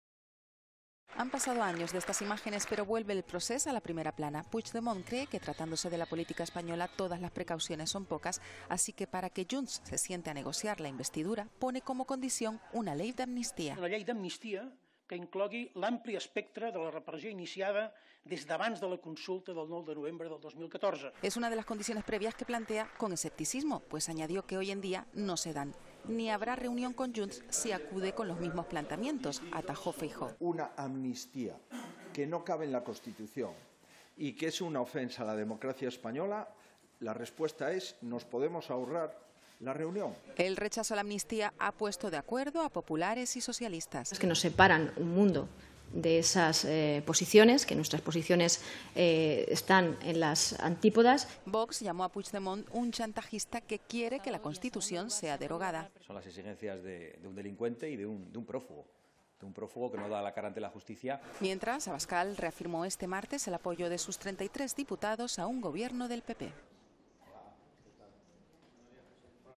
Puigdemont pide una ley de amnistía para negociar la investidura
El expresidente catalán Carles Puigdemont dijo que una ley de amnistía, el reconocimiento y respeto a la "legitimidad democrática" y la creación de un mecanismo de verificación de los acuerdos son las condiciones previas para que su formación, JxCAT, inicie las negociaciones de investidura del próximo Gobierno de España. Puigdemont señaló que se trata de condiciones "previas", que hoy en día "no se dan".

"Condiciones para / la investidura"